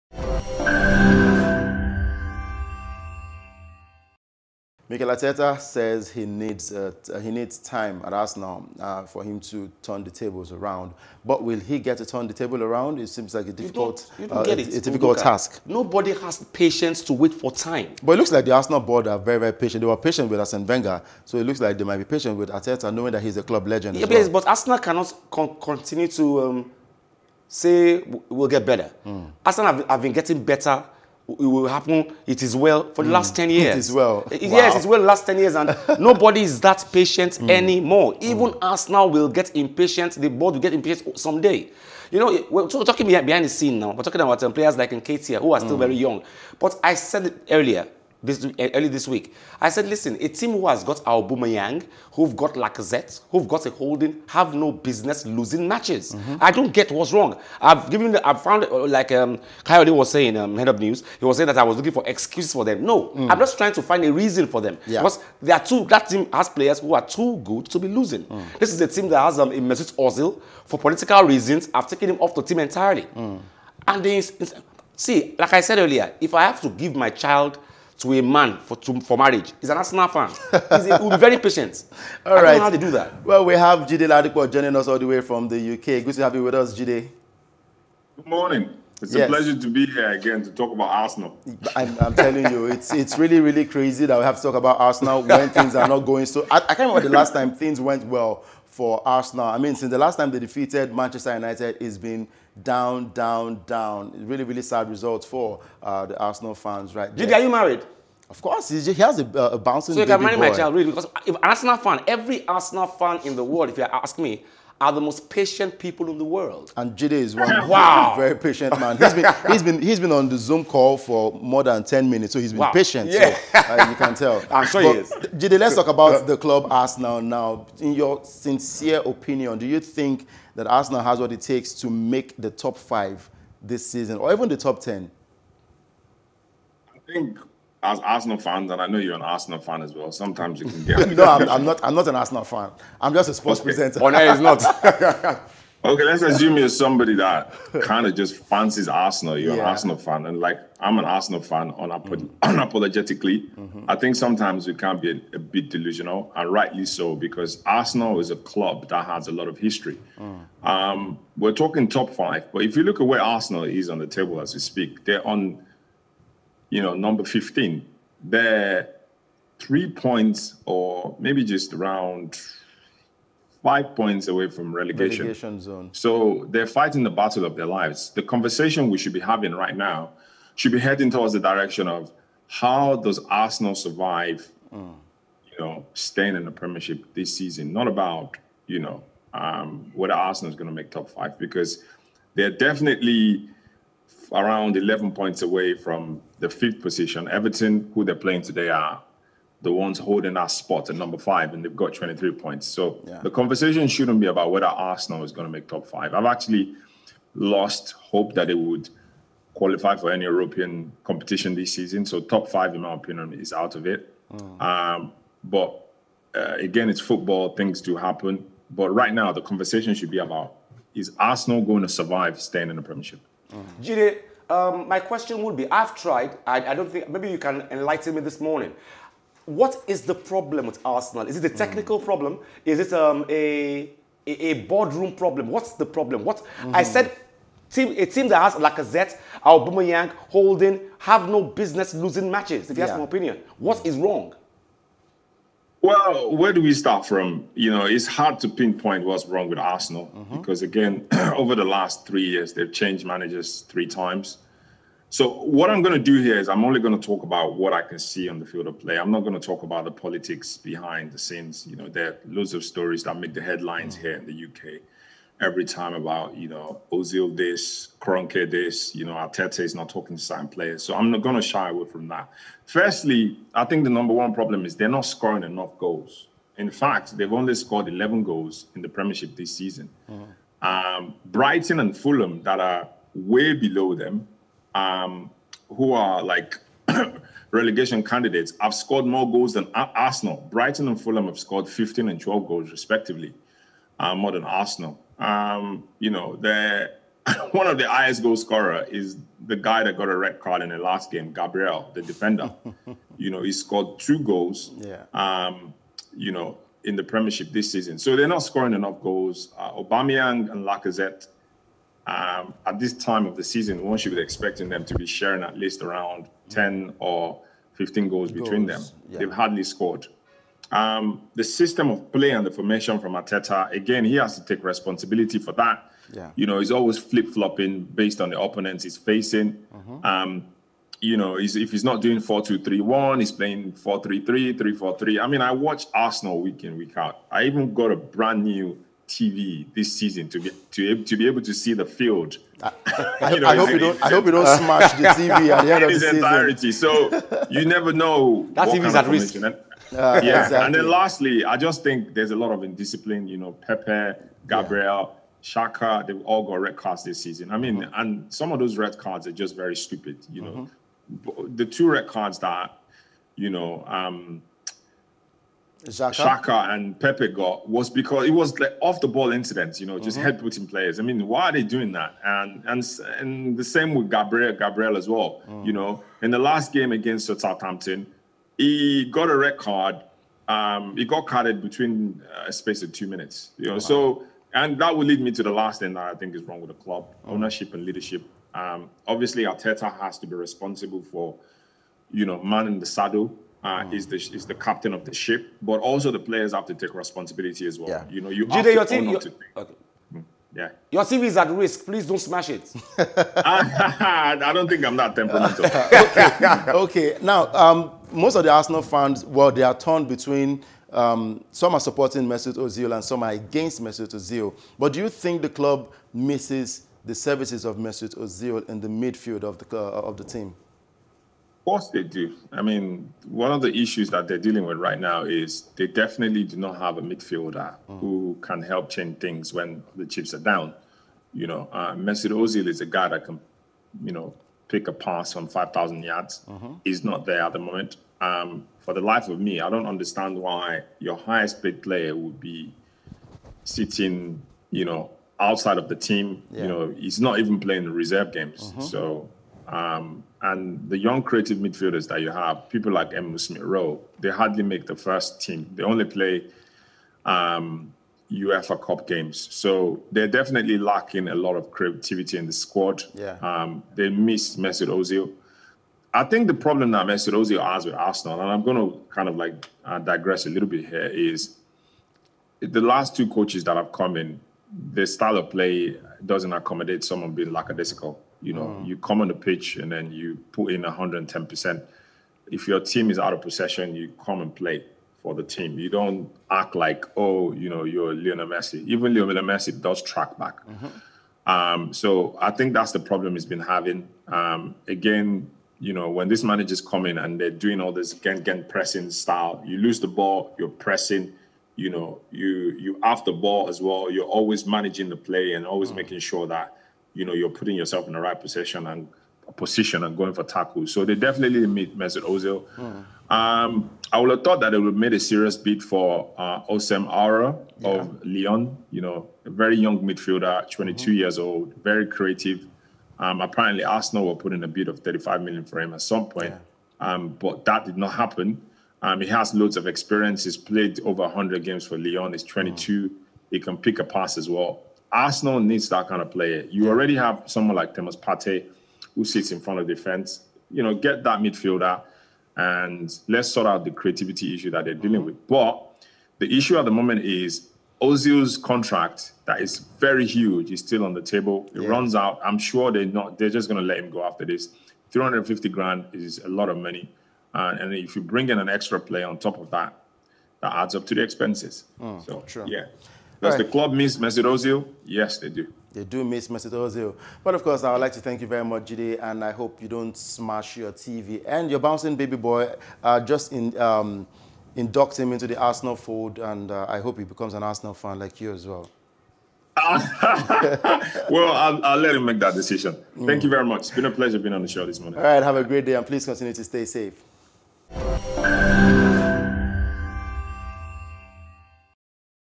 Arsenal's Crisis Lingers
On Plus Sports, we looked at the North London Club; Arsenal and how things have gone at the club.